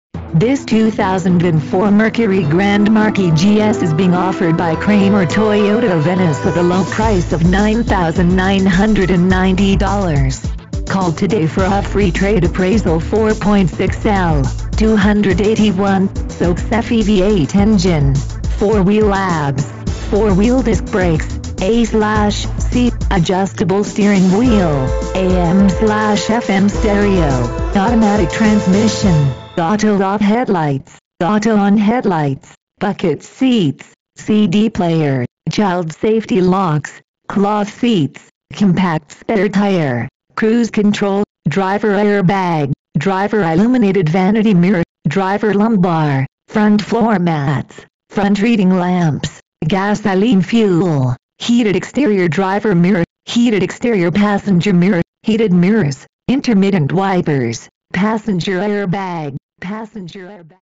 2004 Mercury Grand Marquis GS Venice FL
Price: $ 9,990
Mileage: 54,764
Body Style: Sedan
Ext. Color: Green
Int. Color: Beige
Engine: 8 Cylinder Gasoline
Drive Type: 2 wheel drive - rear
Fuel Type: Gasoline
Doors: Four
Stock No.: 91543B
VIN: